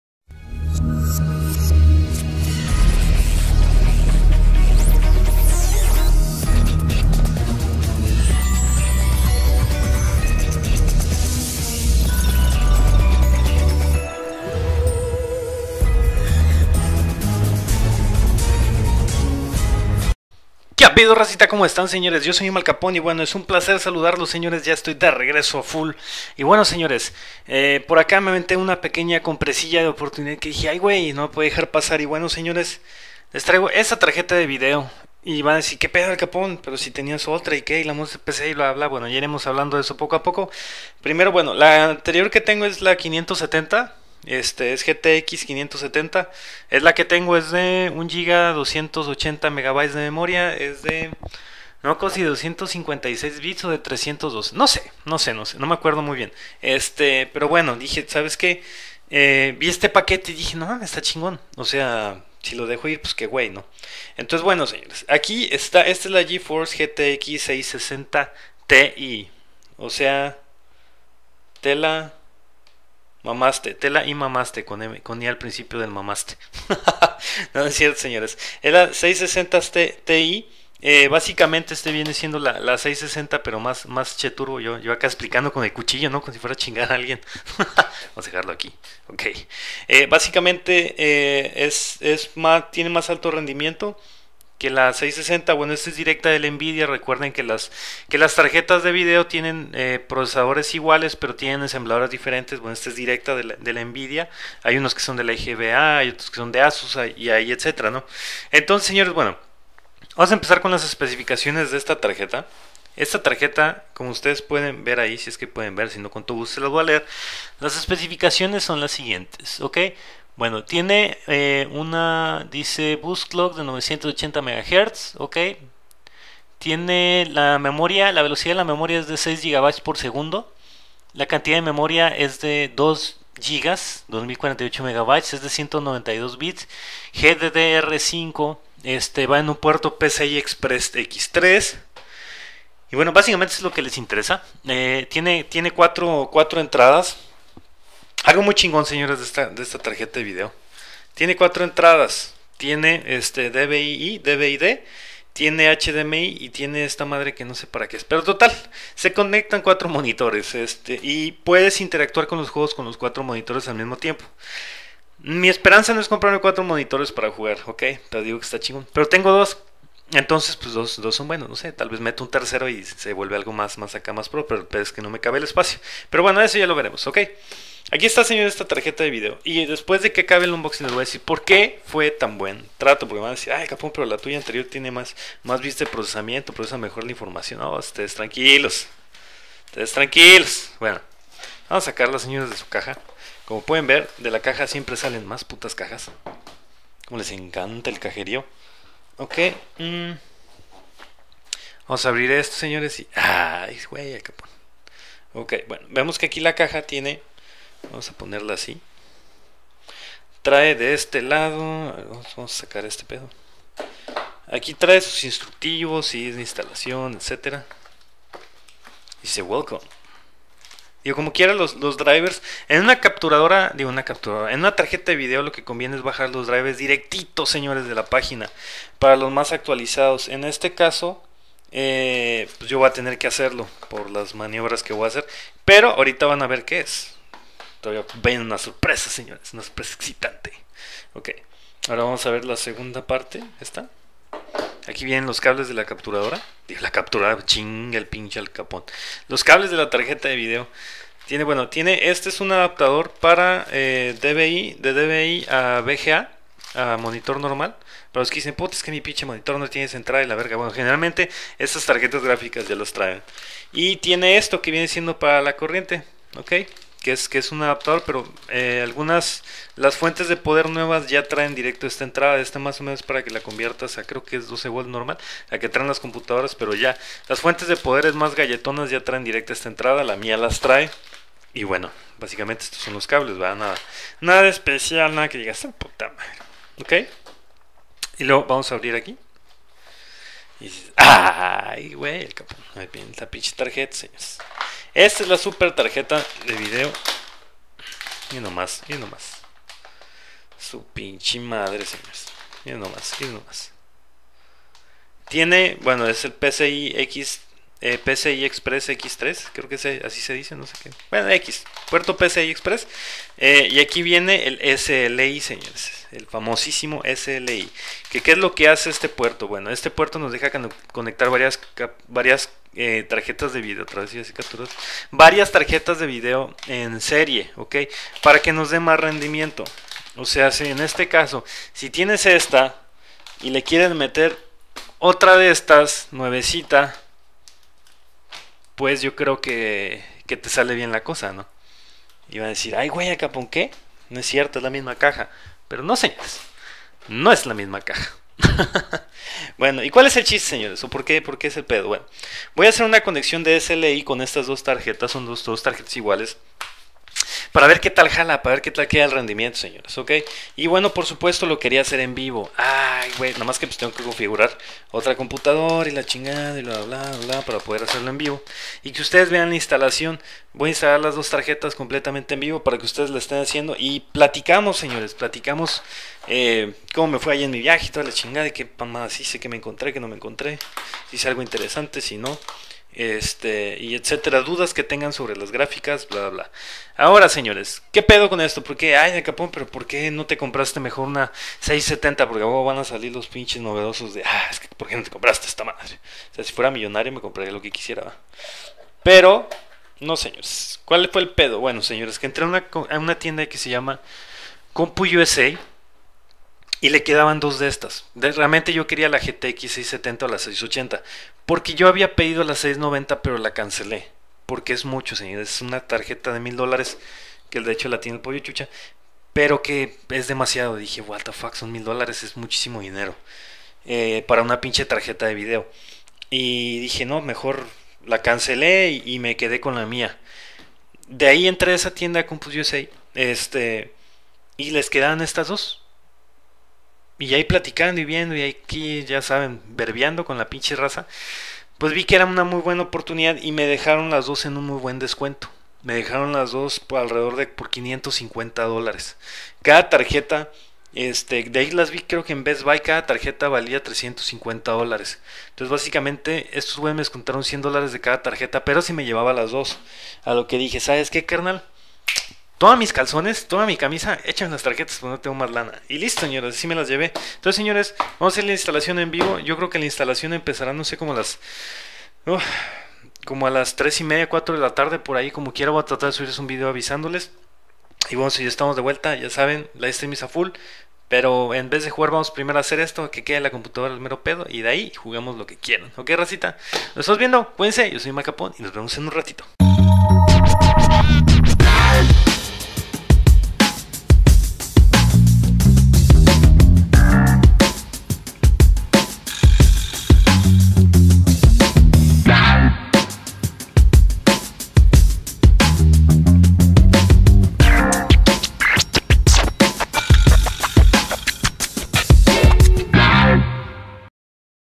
Unboxing Nvidia GeForce GTX 660 TI para SLI!!
Que tal Razita, en un rato va a ser platicando e instalando en vivo con la razita!!